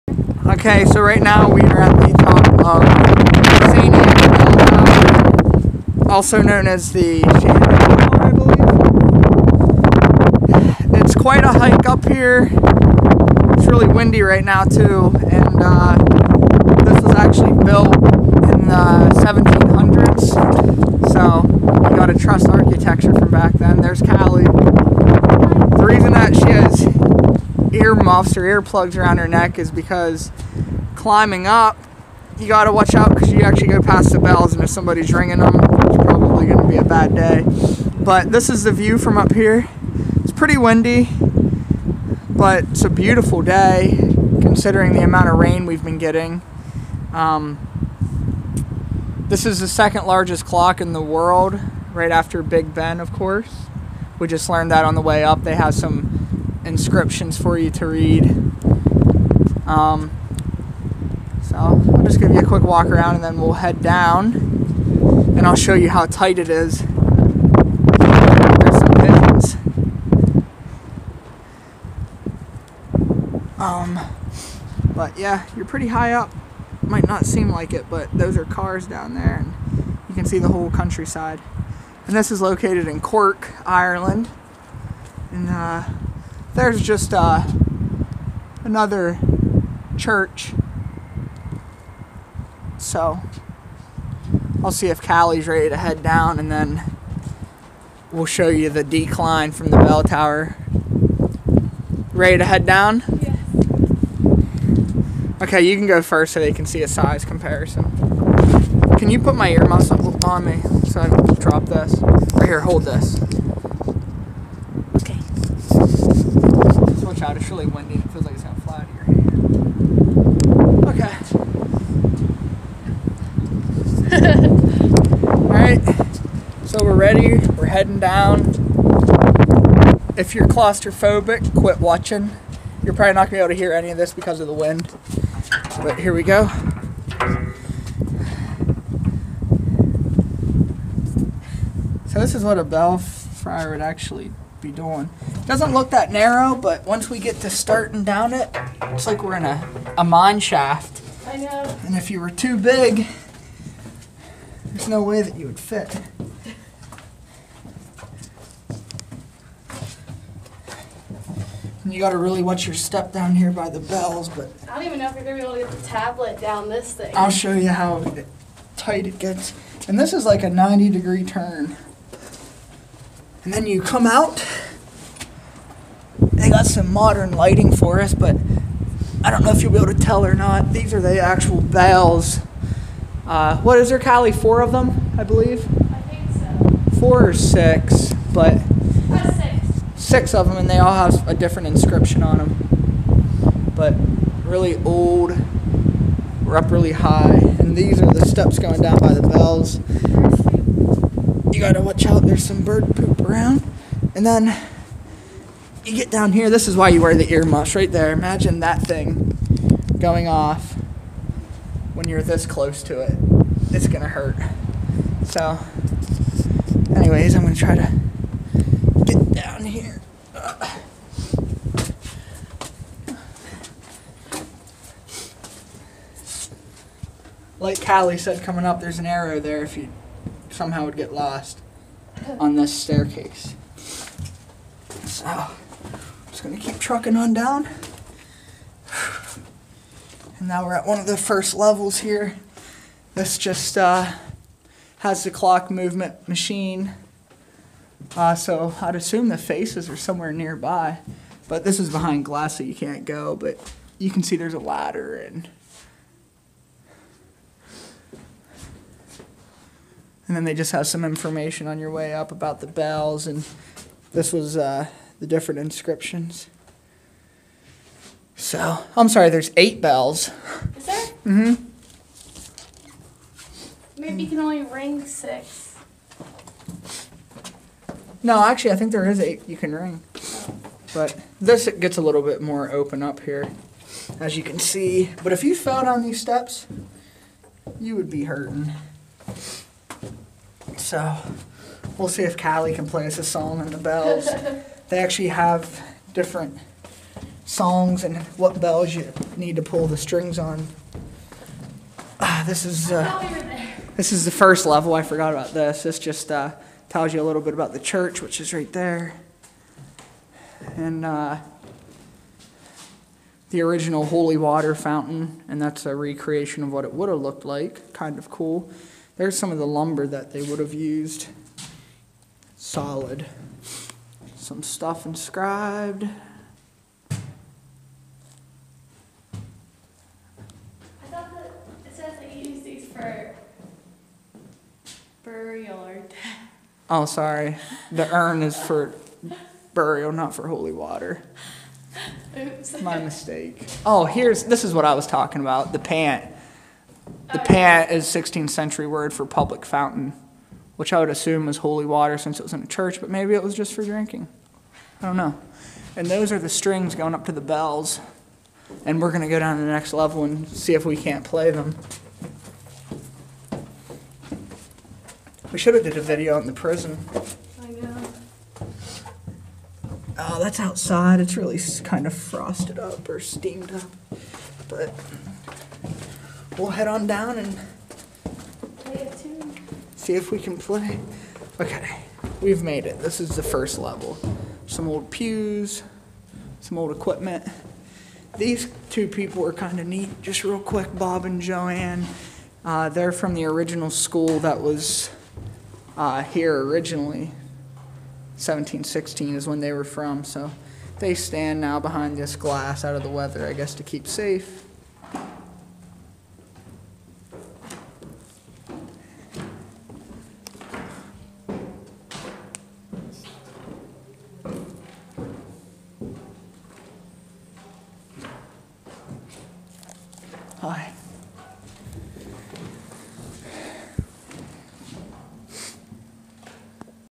Shandon Bells
Shandon Bells Ireland. . .Sorry you don't get to hear the playing of the bells someone came up so I stopped recording.